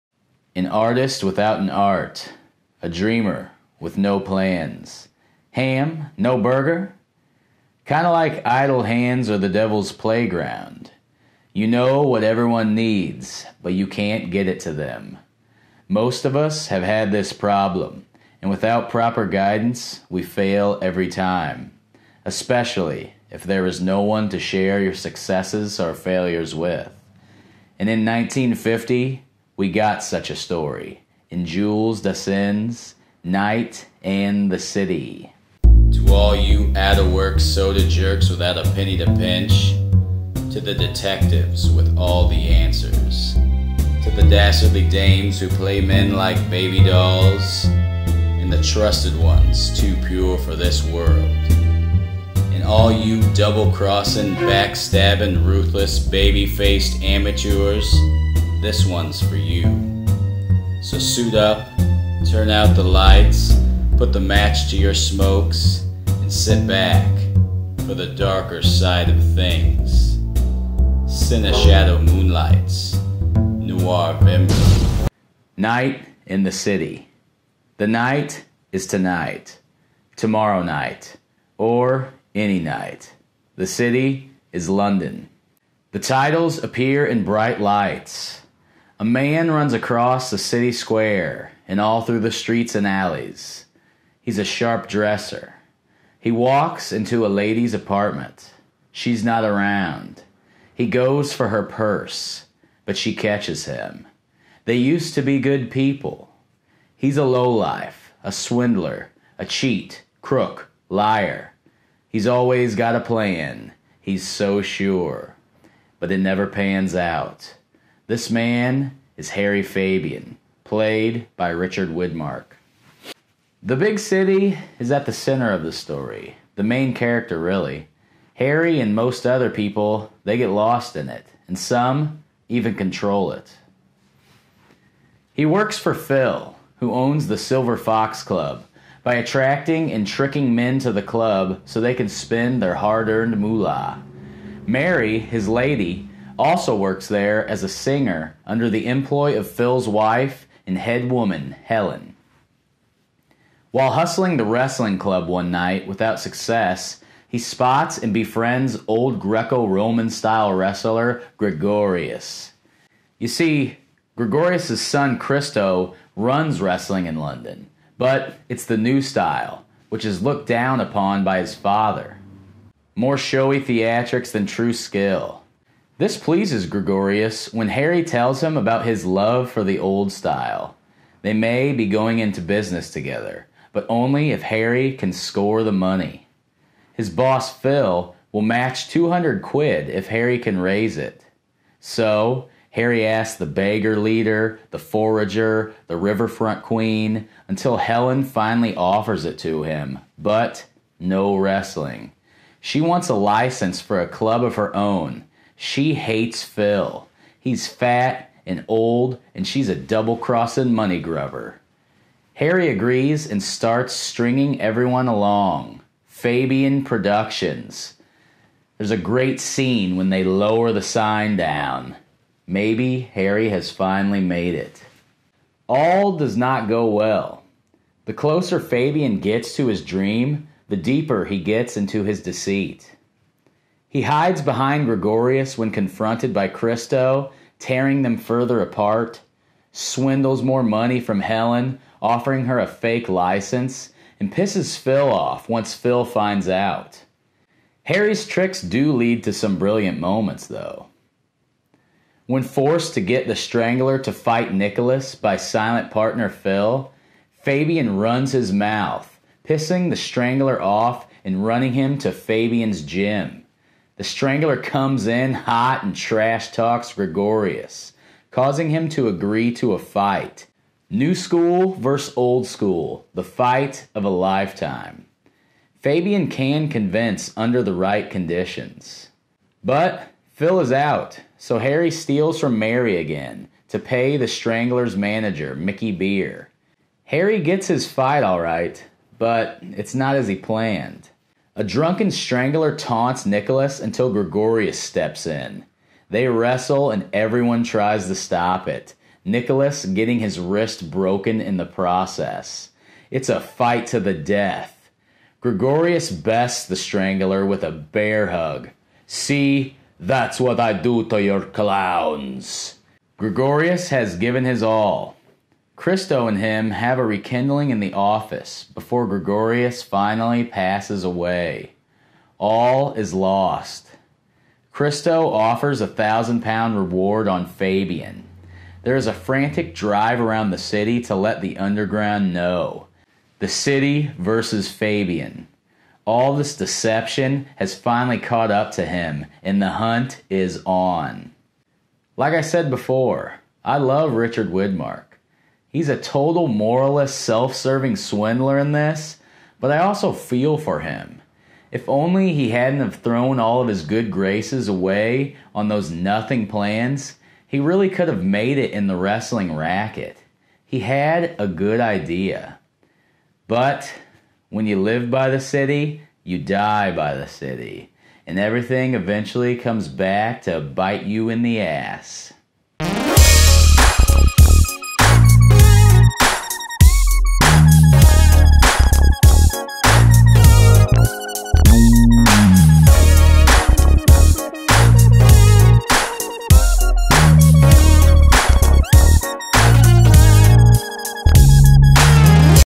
Noirvember 2019 #22 - (Night and the City - Jules Dassin - 1950)
Day 22. This is the start of something special... or not. We go through the history of film noir, all through the month of November.